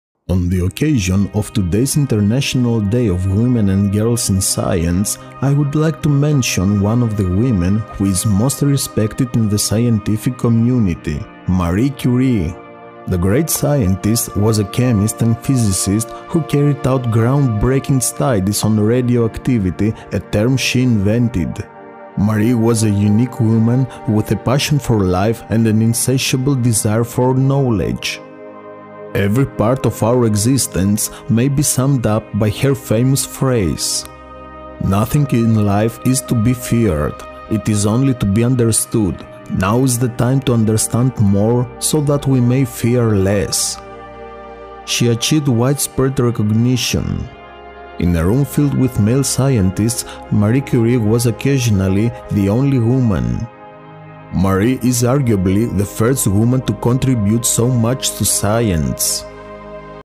MARIE CURIE - The most brilliant female scientist!
On the occasion of today's International Day of Women and Girls in Science, I would like to mention one of the women who is most respected in the scientific community - Marie Curie!
The great scientist was a chemist and physicist who carried out ground-breaking studies on radioactivity - a term she invented!

Credits:
The original uploader was Starwarsbuffyccg at English Wikipedia. - Transferred from en.wikipedia to Commons by IngerAlHaosului using CommonsHelper., FAL,